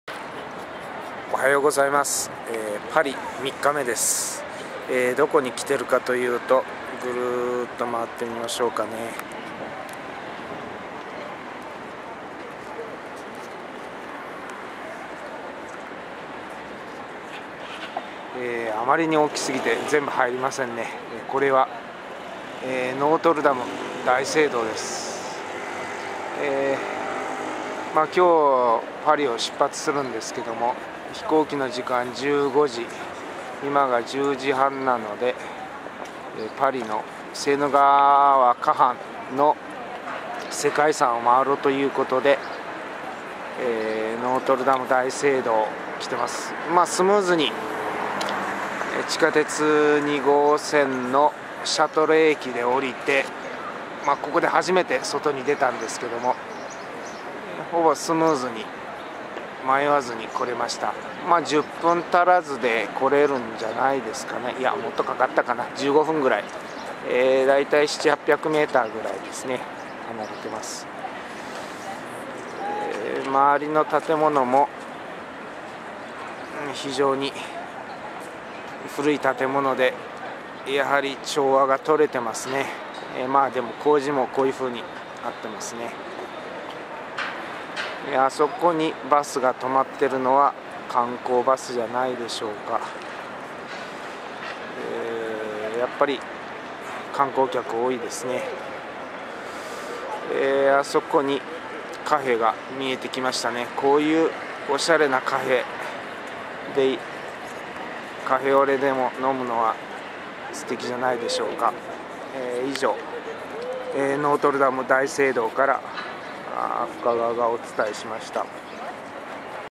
Paris ,La Seine Cathedrale Notre-Dame de Paris(*^^)v
花の都、Paris…

全くの田舎もんのオッサンでも憧れますぜっ…
そのなかでも、人気のある世界遺産・セーヌ河岸…
初期ゴシック建築のノートルダム大聖堂(*^^)v

その圧倒的な存在感に感動ですっ